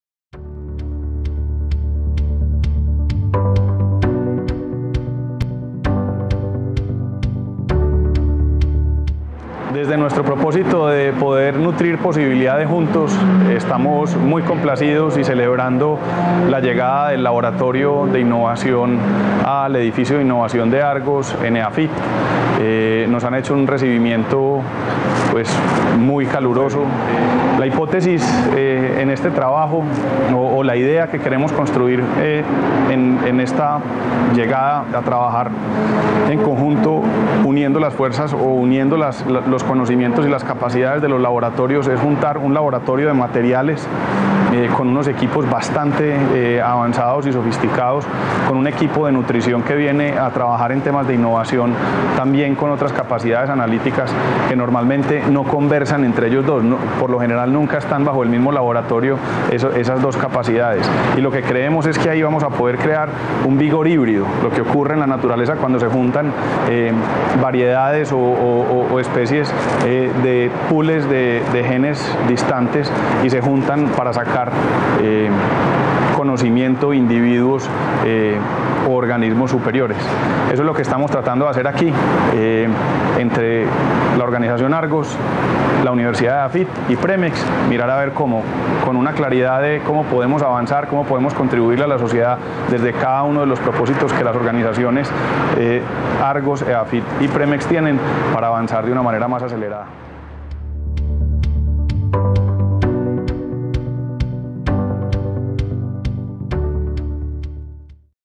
Alejandro Mesa: ¿Por qué es importante unir capacidades para construir conocimiento?
Argos, Premex y Eafit unieron sus capacidades en el Laboratorio Biotecnológico del Centro Argos para la Innovación. Alejandro Mesa, presidente de Premex, nos dice porqué es importante esta unión de capacidades .